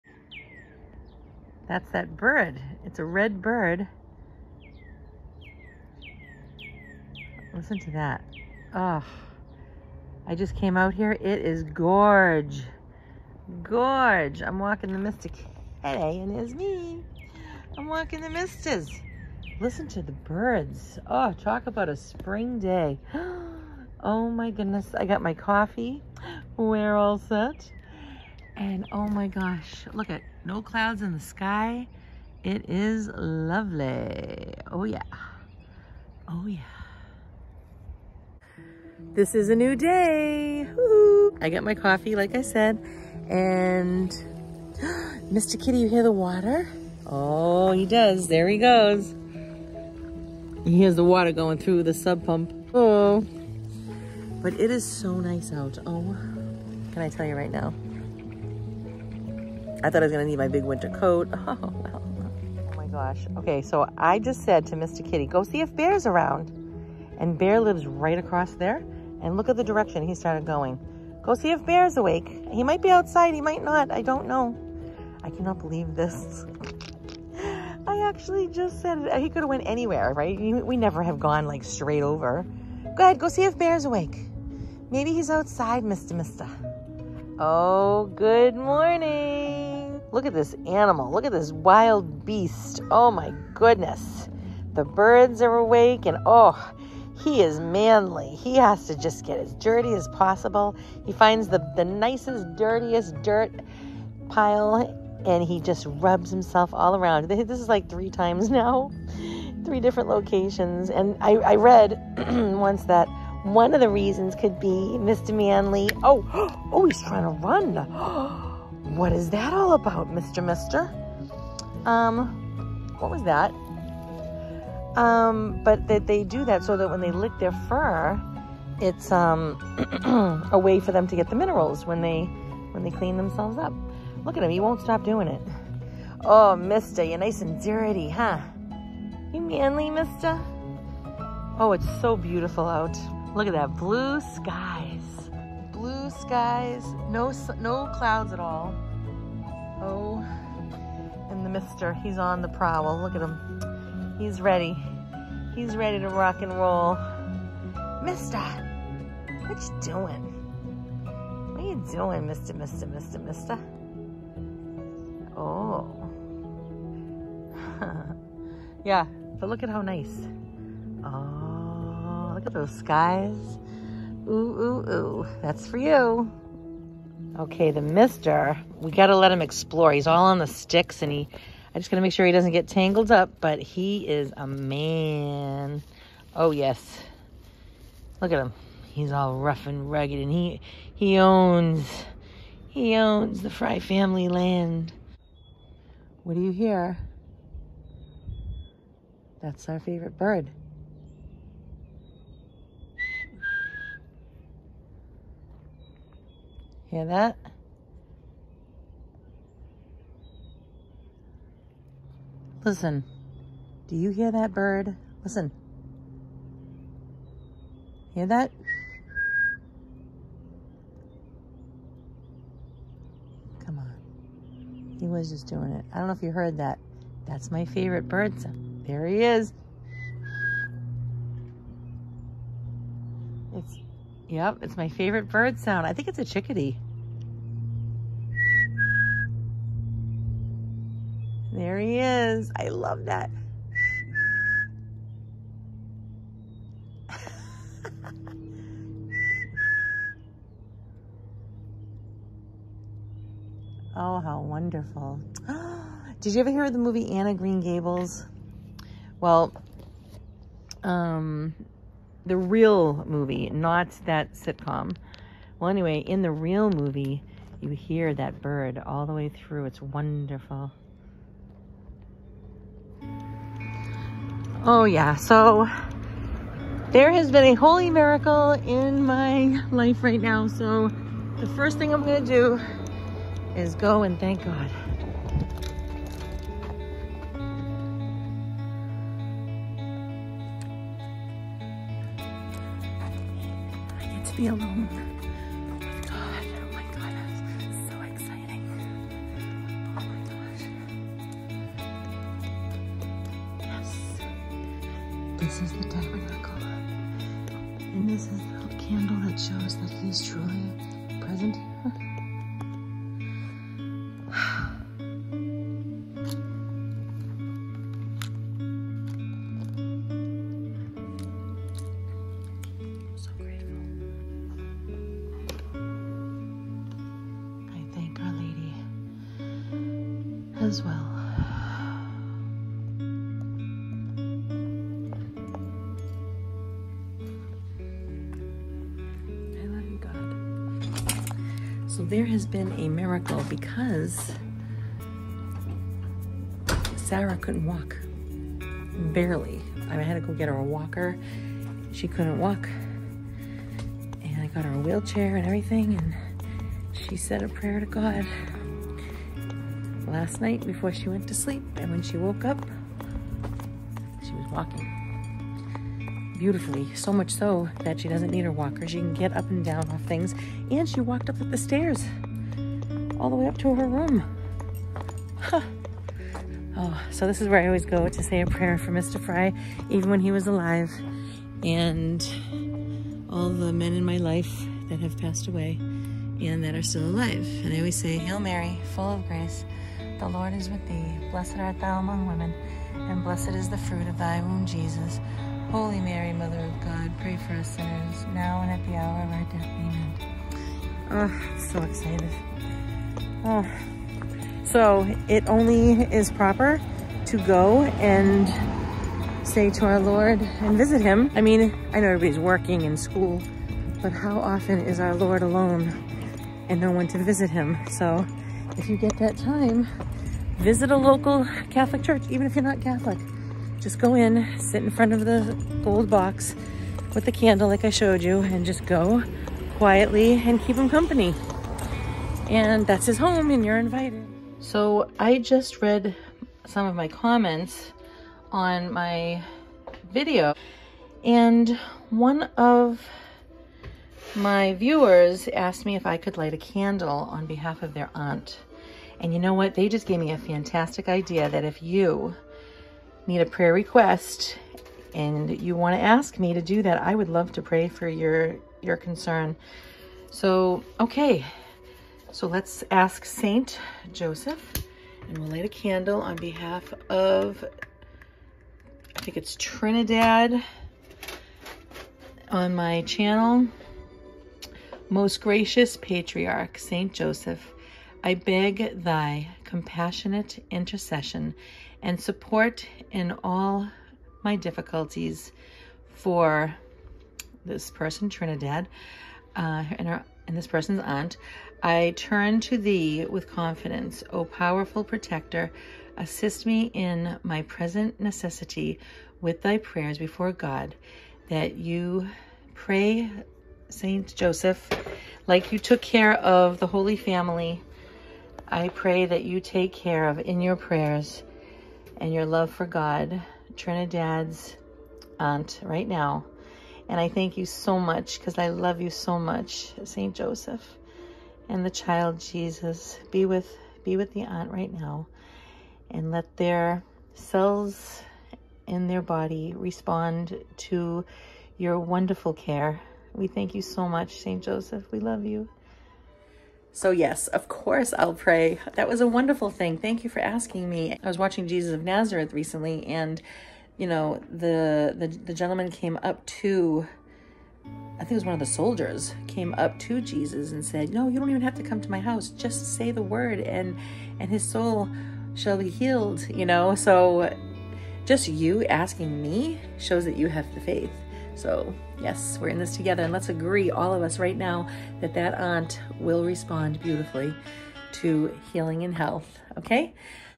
There Has Been a Miracle in Our Family!
There has been so much agony for my beloved little girl watching her go through chemo treatments. I can’t even imagine being her and what she has to endure. It started lately with her legs and she couldn’t walk very well at all because of steroids. Massive amounts of steroids and then all of a sudden I had to take her to the emergency room. I didn’t capture all of that but I’m just letting you know. It left her with weak legs and pain. She said a prayer and this is what happened. Let’s go!￼